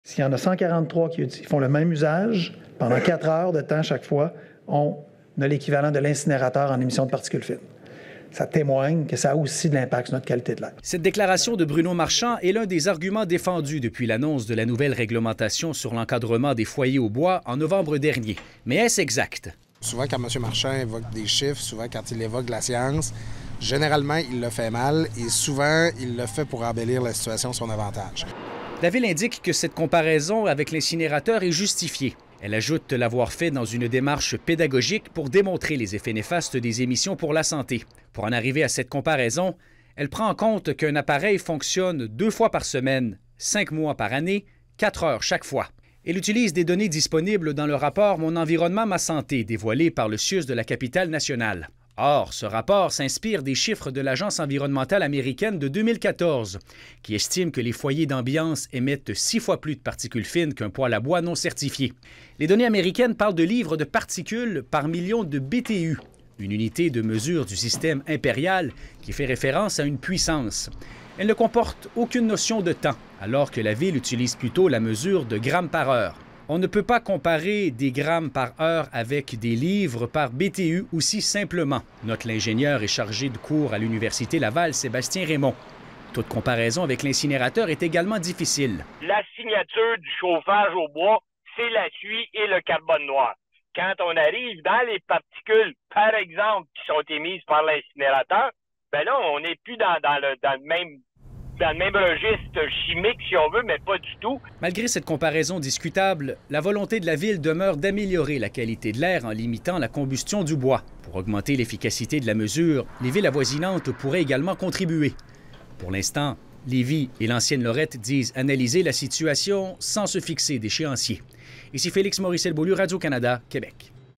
143 foyers au bois polluent-ils réellement autant que l’incinérateur?
La Ville de Québec prétend que 143 foyers d’ambiance émettent autant de particules fines (PM2,5) que l’incinérateur. Or, une conversion d’unités impériales en unités métriques vient fausser le calcul utilisé pour arriver à ce résultat.